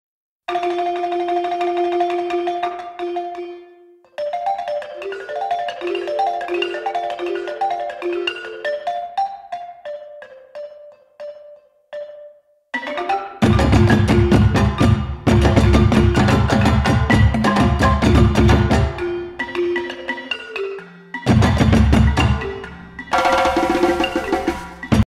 Chinese Character 祭 Ritual
1102. 祭 Ritual : Japanese Kanji (Chinese character) composed of:
Flesh Again = ( 月 Flesh + 又 Again )
Radical : 示 Show

Readings
Kun: まつ.る、 まつ.り、 まつり
On: サイ

Phonetic: ? (?)

ritual, offer prayers, celebrate, deify, enshrine, worship